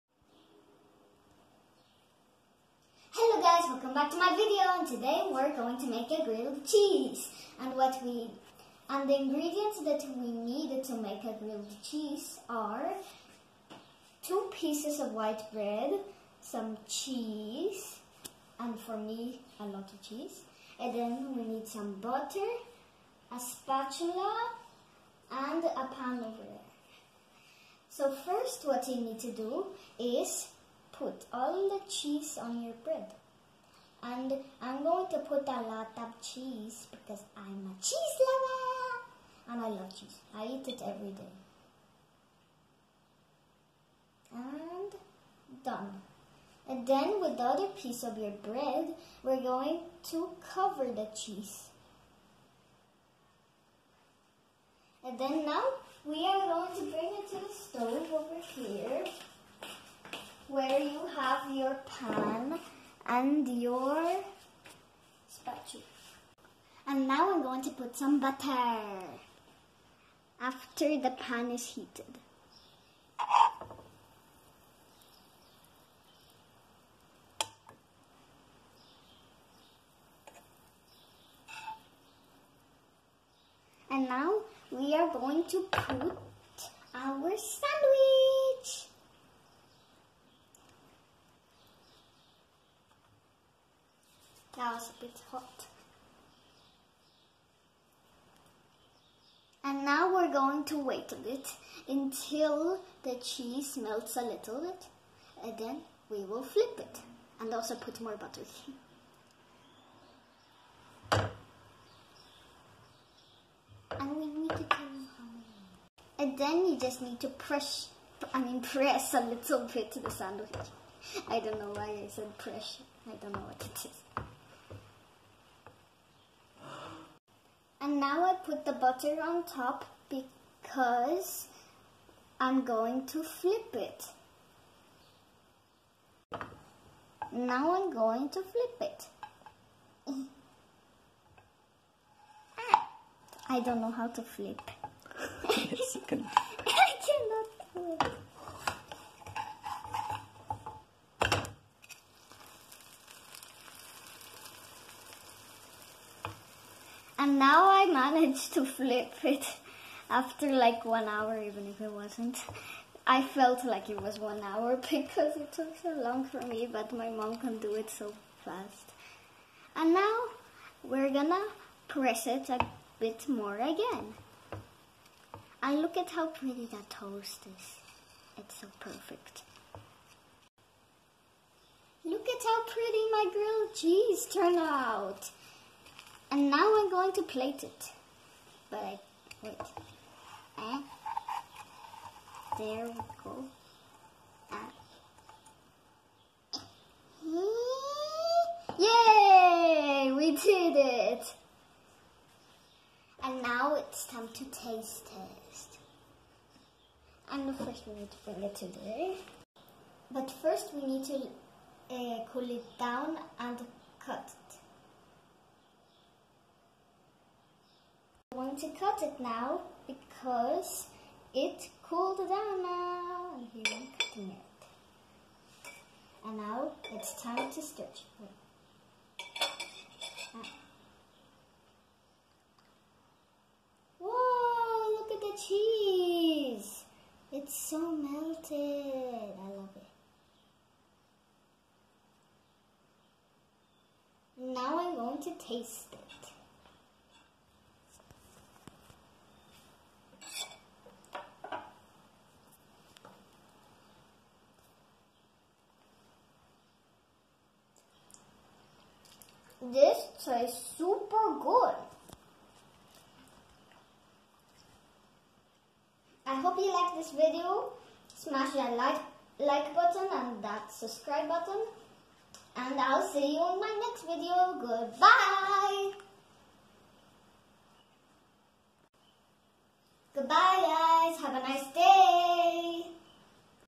My daughter's Best Grilled Cheese ever❤️ 🧀🧀🧀
The first time she made her own grilled cheese. It was a success. Yay!!👏👏❤️
Excuse our dirty stove and pan we just suddenly think of making this video 🙄😂Thanks everyone ❤️